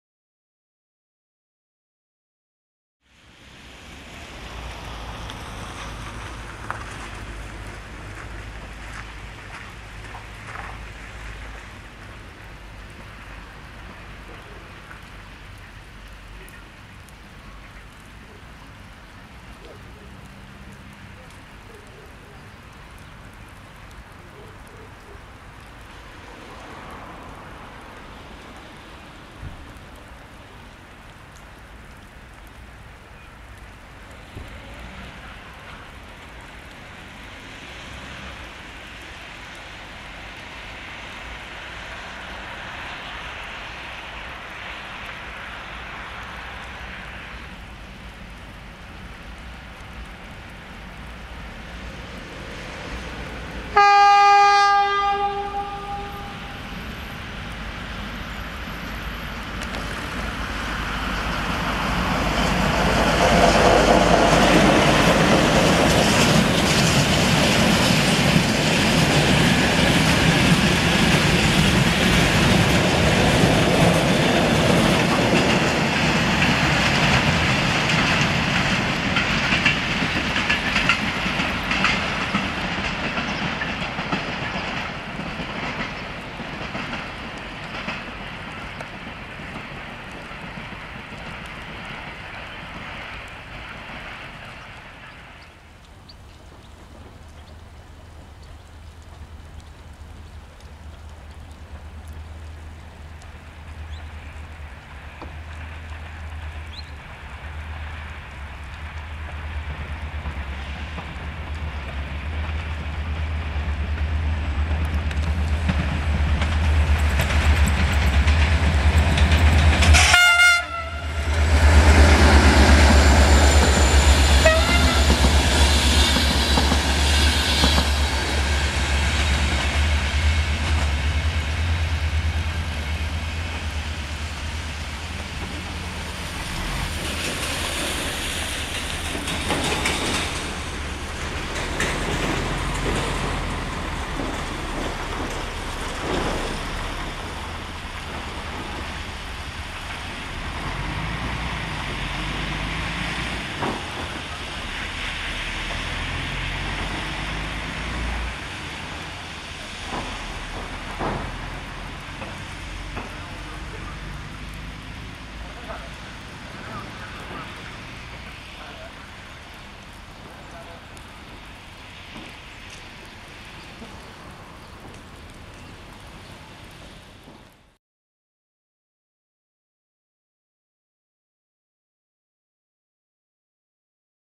Two Trains Trough Snow in Oradea Est
Two Trains from Snow in Oradea Est
Doua Trenuri prin Zapada in Oradea Est

1:Marfar - CFR - LDH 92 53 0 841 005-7
2:IC532"Avram Iancu" - Oradea - Bucuresti Nord - Jimmy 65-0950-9
3:Video Ending

© Train97